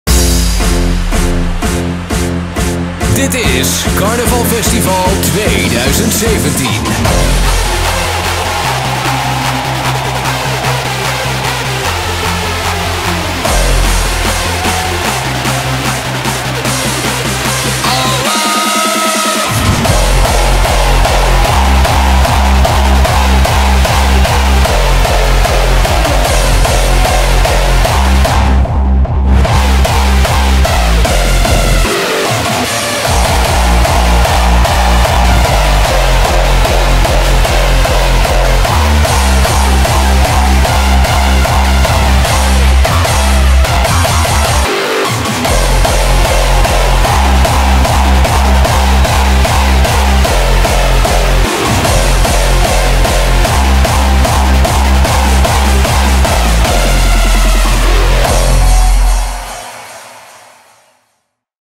Karnaval Festival 2017 - Video Recap
Wil je tijdens carnaval gewoon lekker gas geven? Geen hoempapa-bandjes, maar podia en wagens met dikke geluidssystemen, een dj-booth en gaan met die banaan! Karnaval Festival is een uniek hardstyle-festival met een carnavaleske thema.